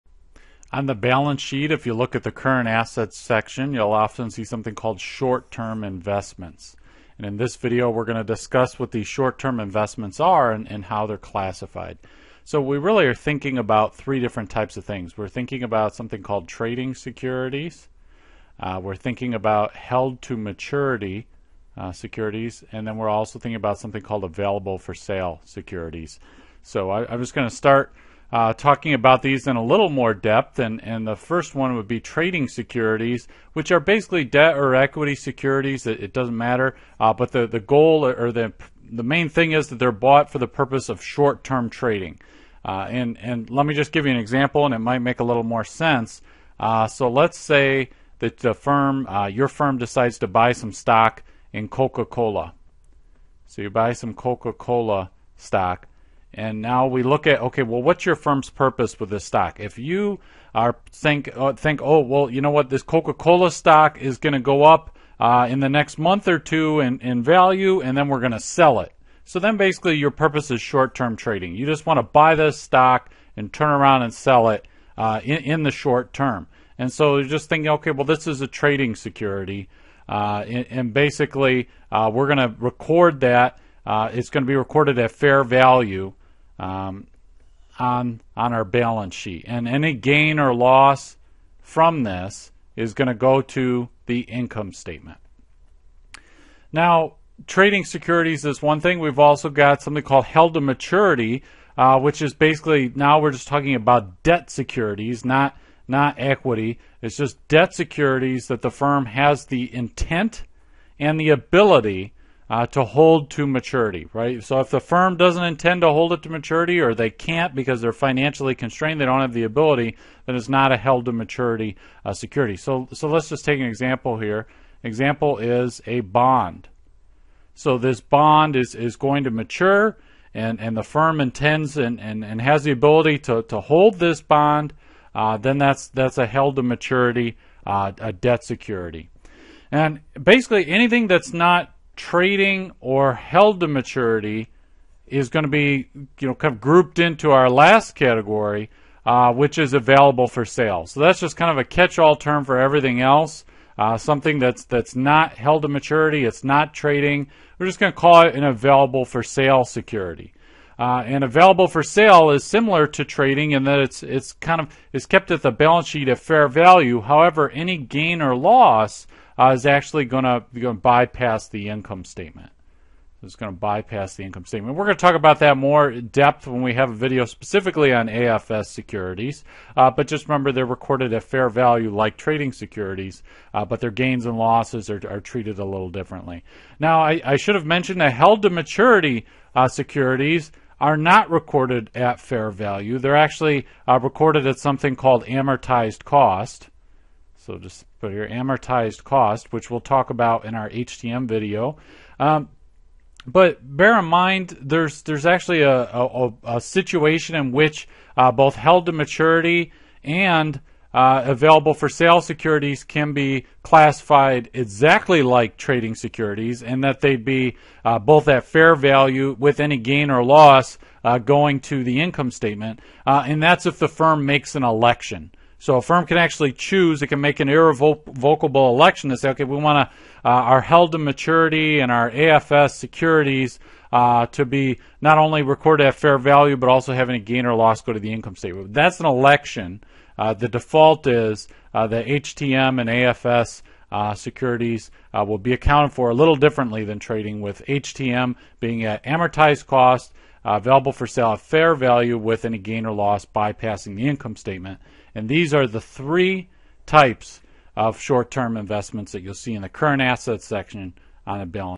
Short-term Investments in Financial Accounting
This video explains what short-term investments are in financial accounting and discusses the 3 types:  trading securities, held to maturity securities, and available for sale securities.
ACCESS INDEX OF VIDEOS
CONNECT WITH EDSPIRA
CONNECT WITH MICHAEL
ABOUT EDSPIRA AND ITS CREATOR